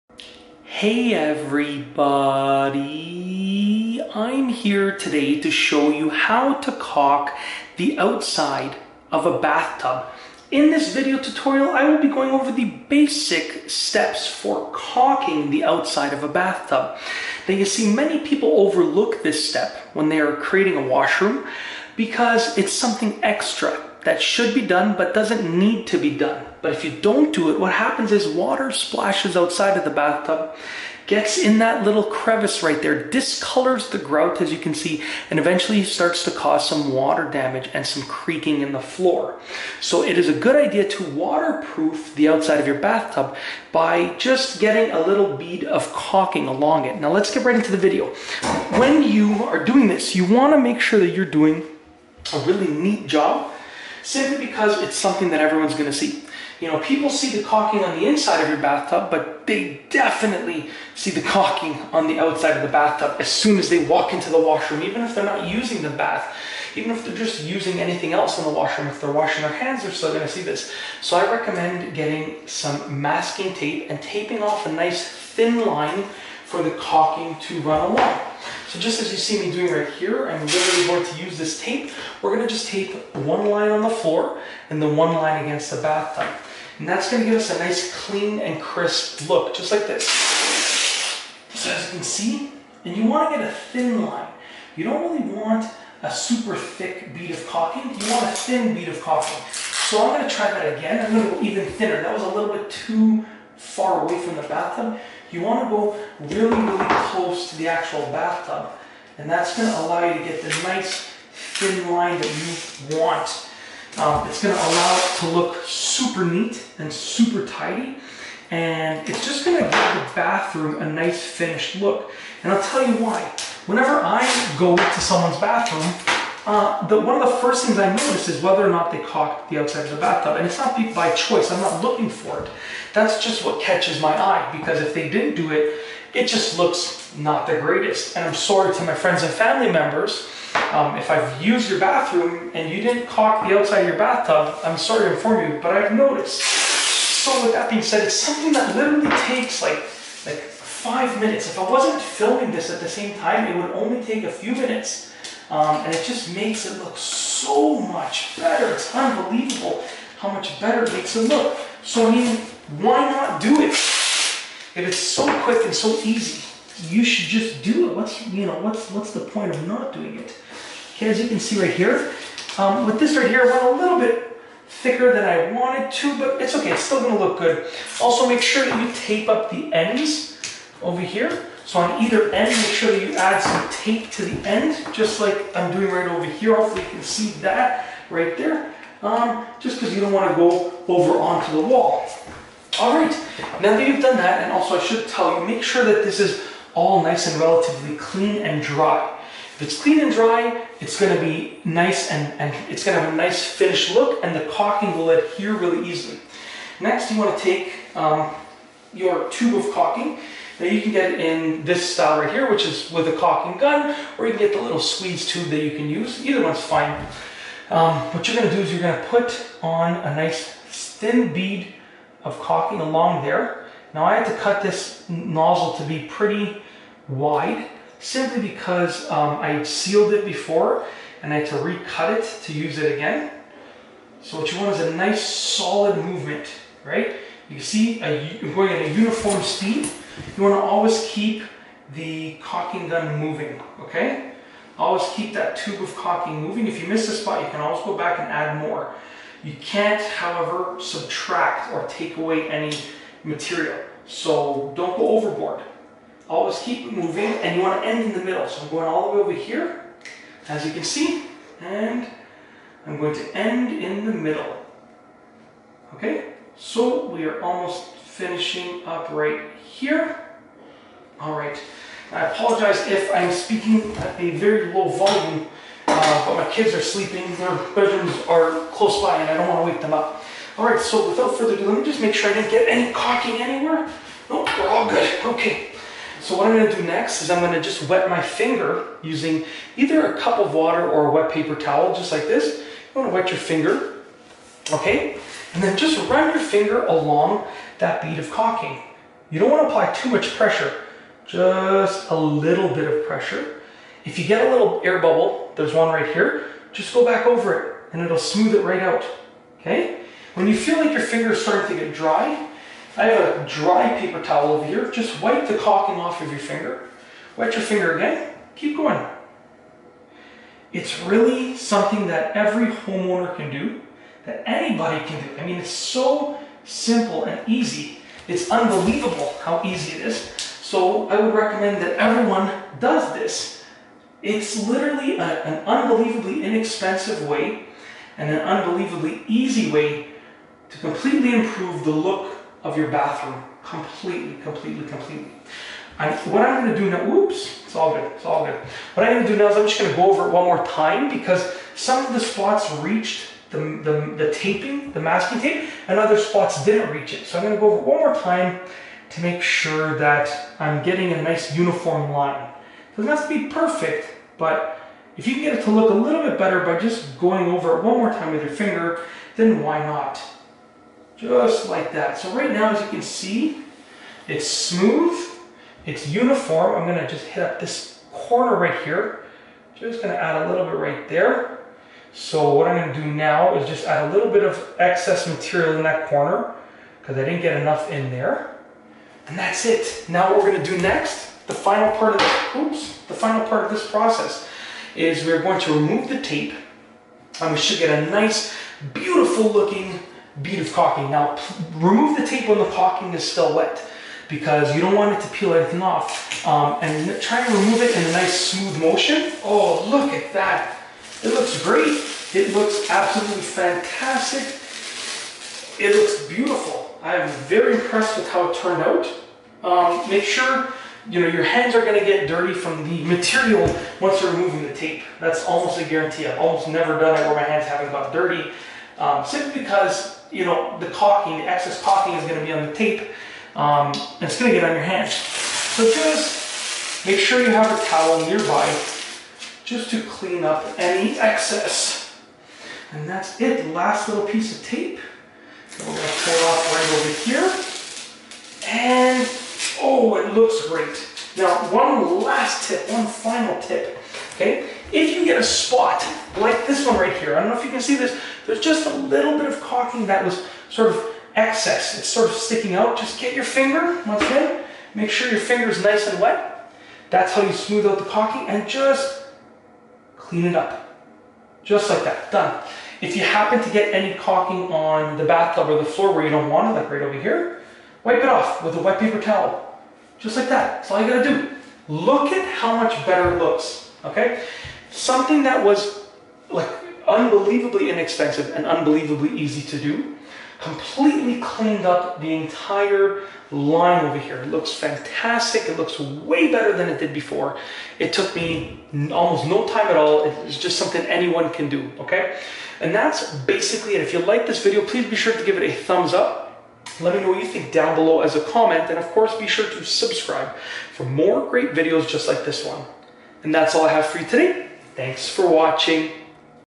How To Caulk The Outside Of A Bathtub-Tutorial
In this video, I show you how to caulk the outside of a bathtub. It is actually really easy to do and will make it look a lot better. In addition, it will protect the outer edges of the bathtub and tiles from water damage, which is something that you definitely want to avoid. It only requires some basic tools and will not take a long time. And if you use some painter's tape, just like I show you, your caulking line will look absolutely amazing!